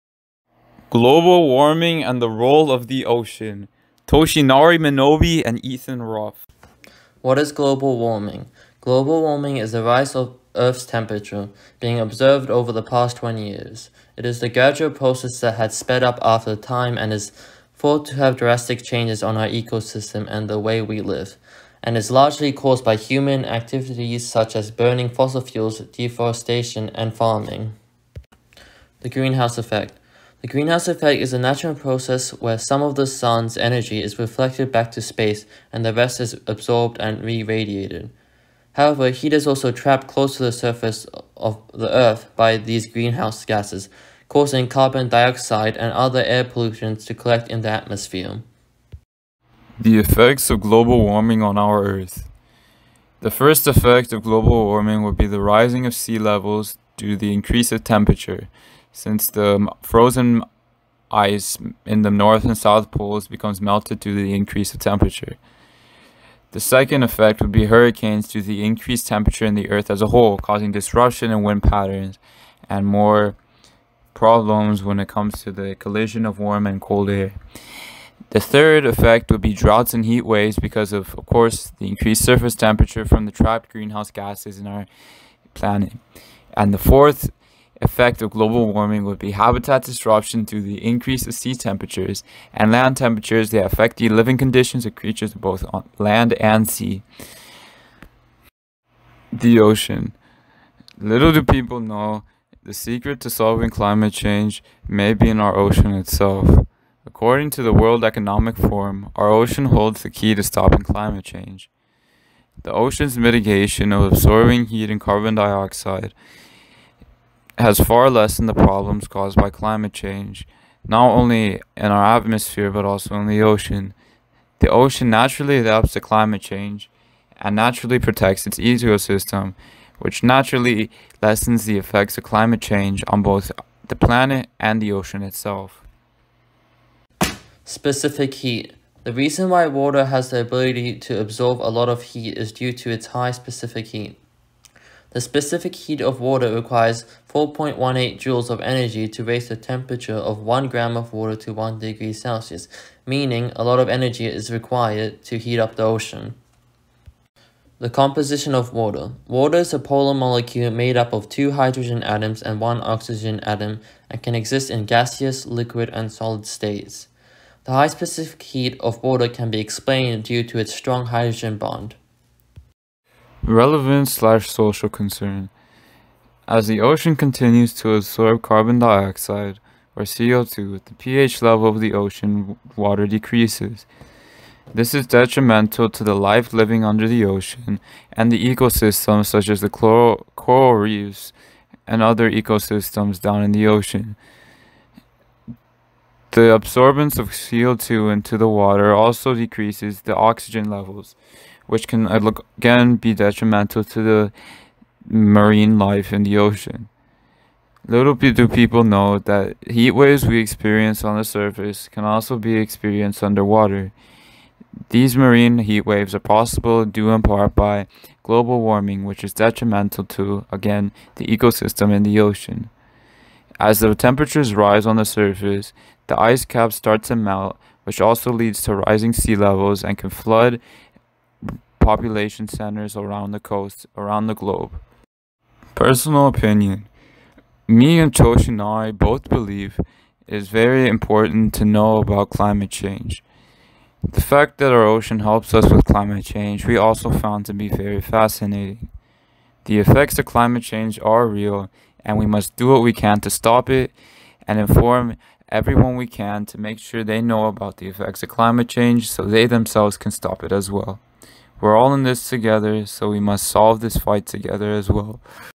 Toshinari Minobe, Ethan Ruff - Global Warming Project and the Role of the Ocean
STEM Symposium 2020 - MDC West Campus